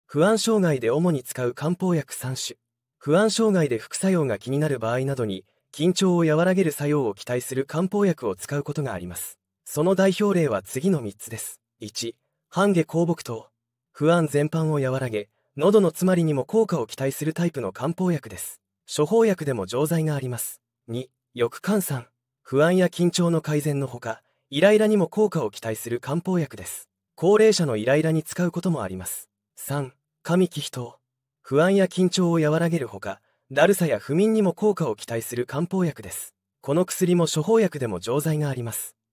不安障害で主に使う漢方薬3種【精神科医監修】半夏厚朴湯｜抑肝散｜加味帰脾湯
「不安障害で主に使う漢方薬3種」について1分でまとめています。
不安障害の治療の中で副作用が心配な場合などに、漢方薬での治療をすることがあります。生薬由来の漢方薬の中で、半夏厚朴湯や抑肝散など、不安の改善に適したものがあります。

監修：春日雄一郎（精神科医、医療法人社団Heart Station 理事長）